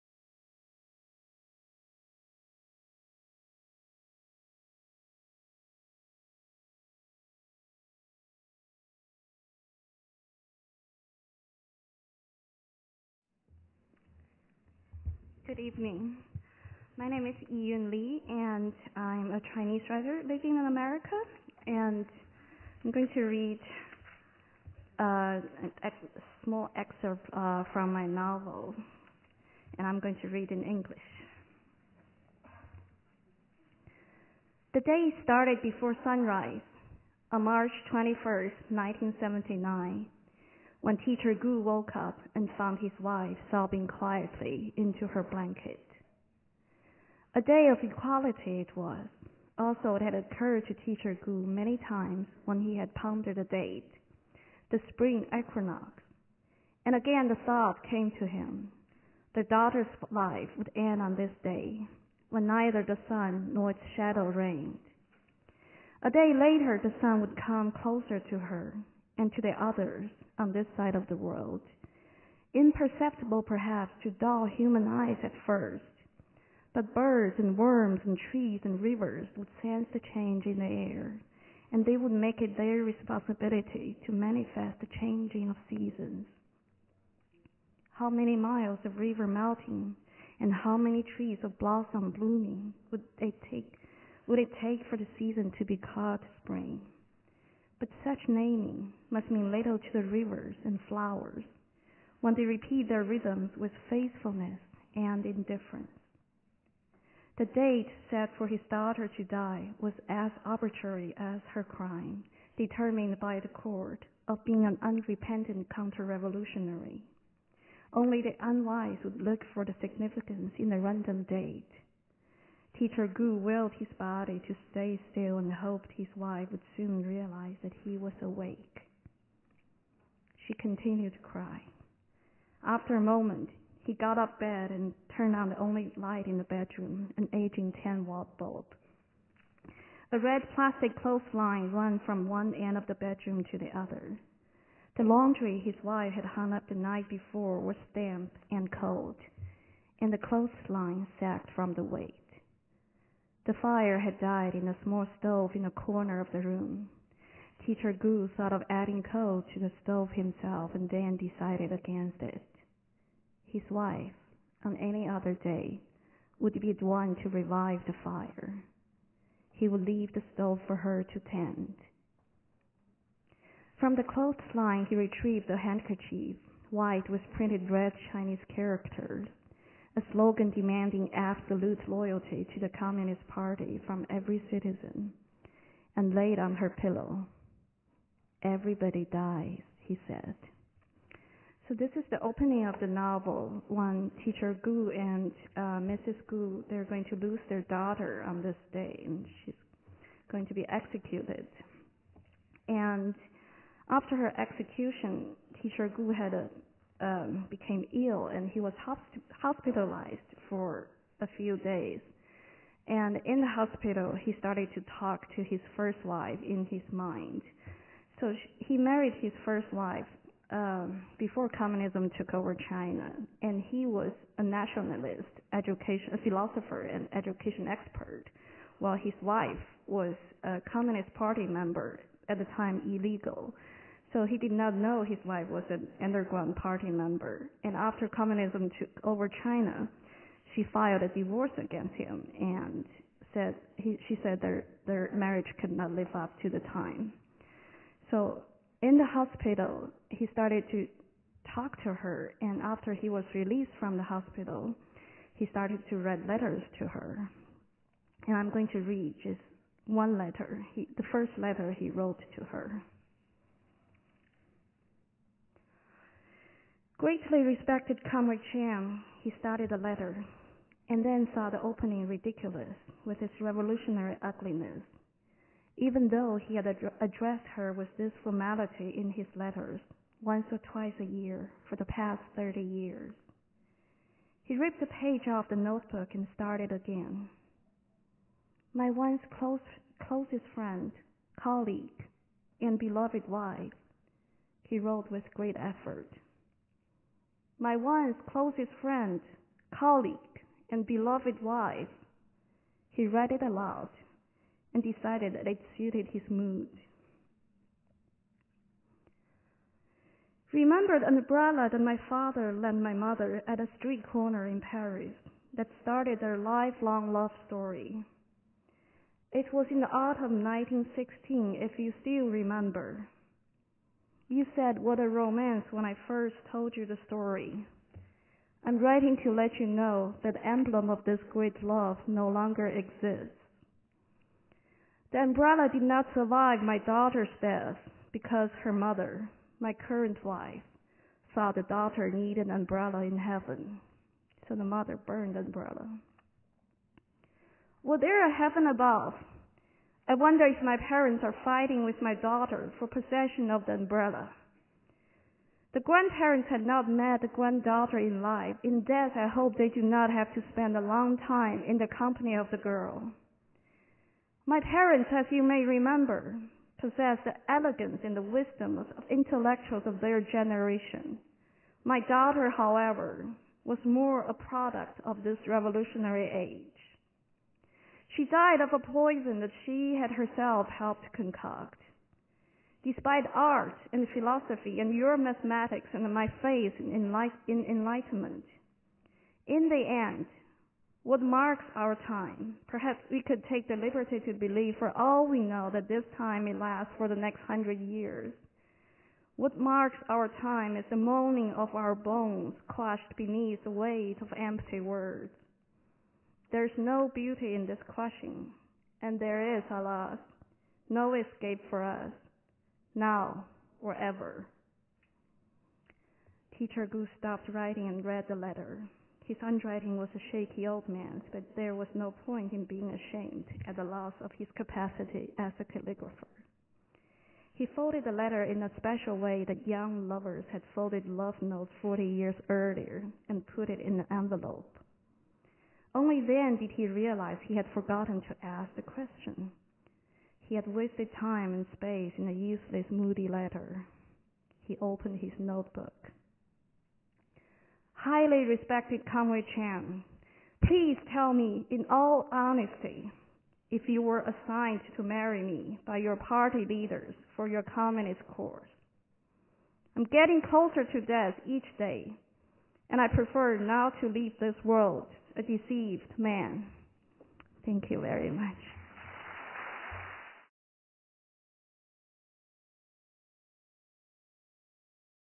Yiyun Li Reads from The Vagrants
Yiyun Li reads from her debut novel The Vagrants at Readings From Around the Globe: Opening Night Extravaganza, part of the 2010 PEN World Voices Festival.